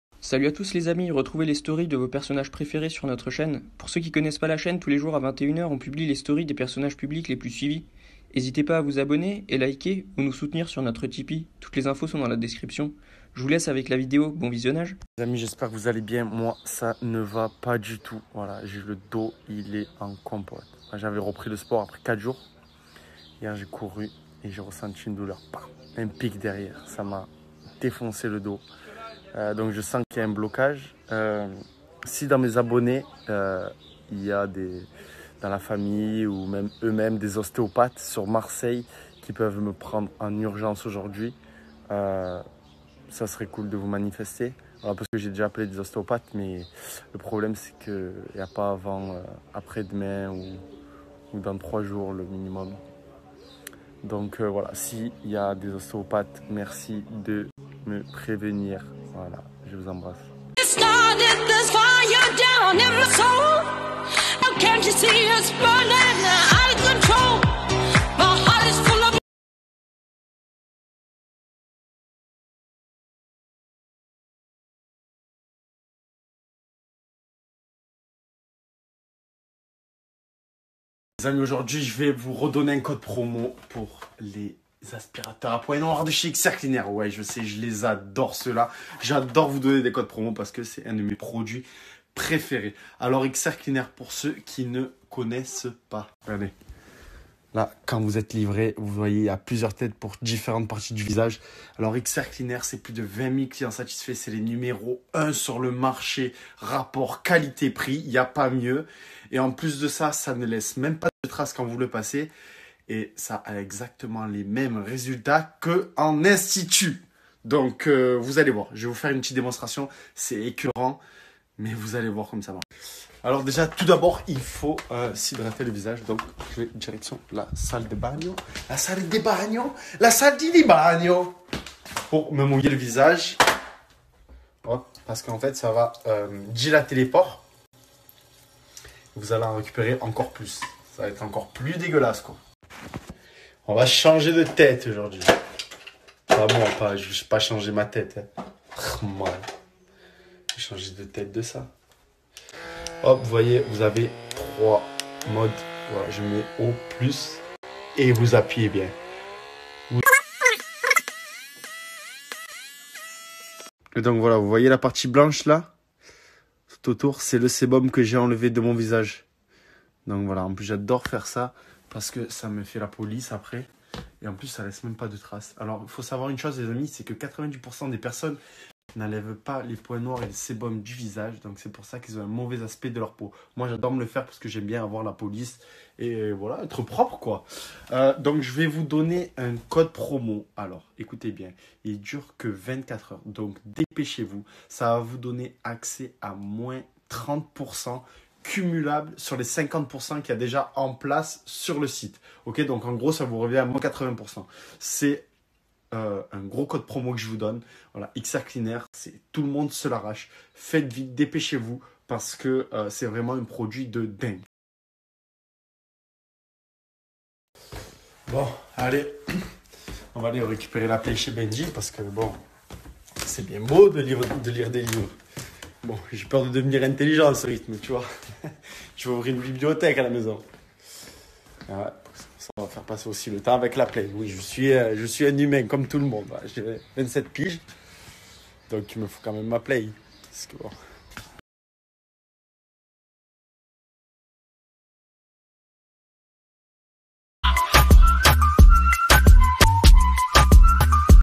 NICOLAS FERRERO 24H STORY INSTAGRAM - 02 Juin 2020
Revivez en intégralité les Story de Nicolas Ferrero le 02 Juin 2020 - Pour plus de story consultez notre playlist

Bon visionnage :)